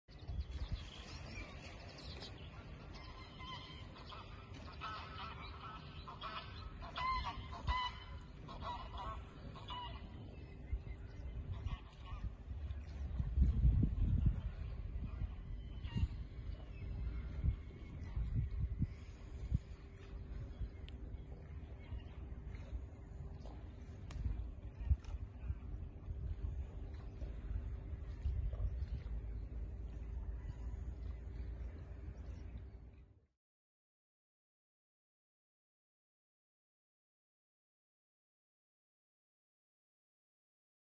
Verharingstijd voor de grijze zeehonden I Ecomare Texel
De grijze zeehonden zitten nu midden in de verharingstijd. Ze liggen daarom veel op het droge. De warmte van de zon verbetert de bloedsomloop, waardoor het verharen sneller gaat. Het verharen gaat niet op alle plekken tegelijkertijd. Daarom zien Annie, Fokje en Betty er een beetje mottig uit. Ze hebben op sommige delen van hun lichaam al een nieuwe vacht en op andere delen nog niet

Wil je meer weten over de zeehonden?